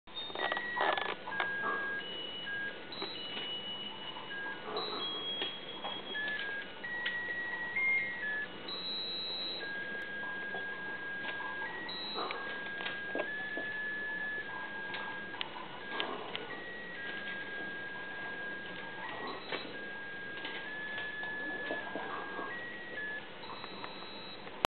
Somali kittens playing with the spinning butterfly on a christmas song...
It can be quite irritating this song but this time it was really nice:) Kittens playing on the penquin christmas song with the spinning butterfly. Almost 12 weeks of age now.